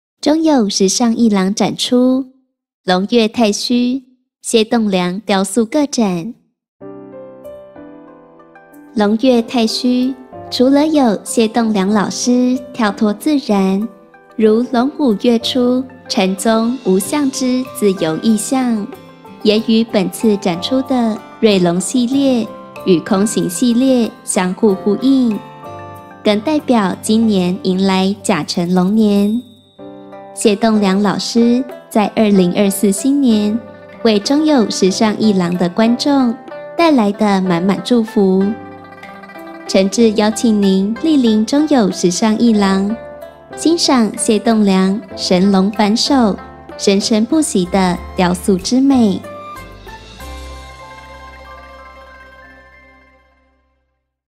《龍躍太虛》謝棟樑雕塑個展｜語音導覽3
龍躍太虛，除了有謝棟樑老師跳脫自然，如龍舞躍出禪宗無相之自由意象，也與本次展出的《瑞龍系列》與《空形系列》相互呼應，更代表今年迎來甲辰龍年，謝棟樑老師在2024新年為中友時尚藝廊的觀眾帶來的滿滿祝福。

中友時尚藝廊以策展者的觀點切入，除了展覽之外，此次也特別規劃打卡牆，標示出謝棟樑老師歷年來於大臺中地區的公共藝術創作，讓觀展者能更加全面瞭解老師的創作脈絡，呈現謝老師更完整的創作歷程。

誠摯邀請您蒞臨中友時尚藝廊，欣賞謝棟樑神龍反首、生生不息的雕塑之美。
中友時尚藝廊 FB粉絲專頁